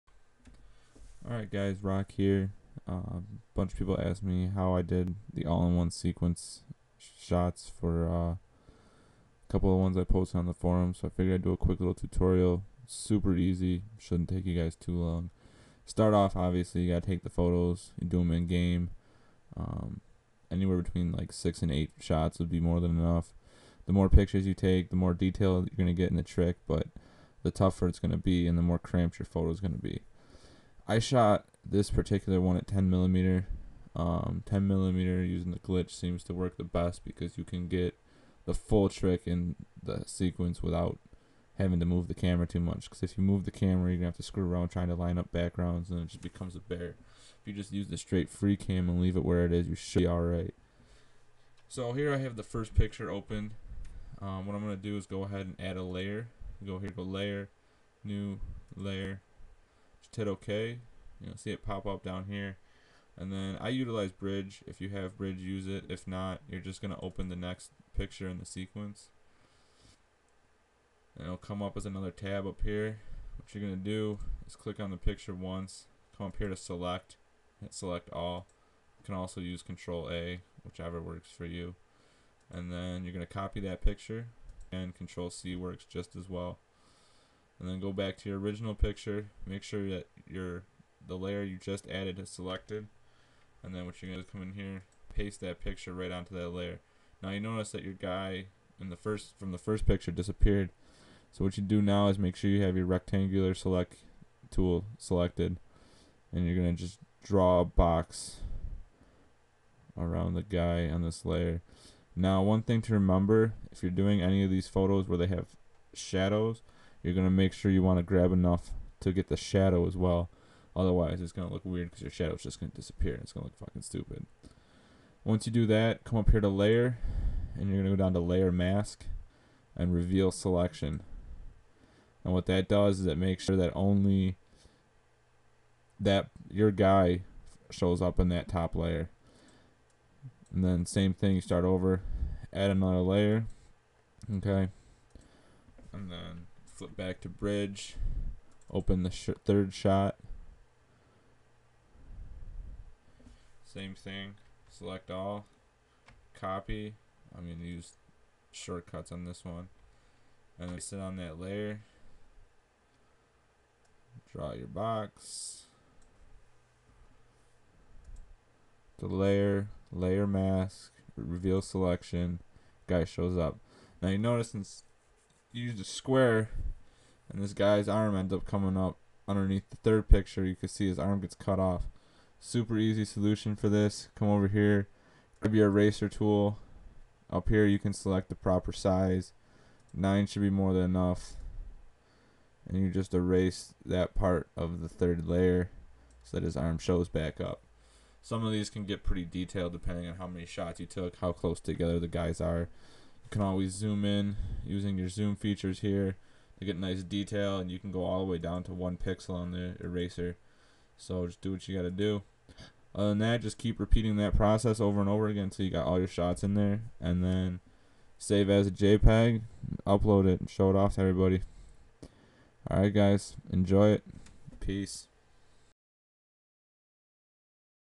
Roccityroller's All-In-One Sequence Photo Tutorial
How to make all in one sequence photos in adobe photoshop CS4. Older versions should work the same way, the only difference is the location of some of the controls.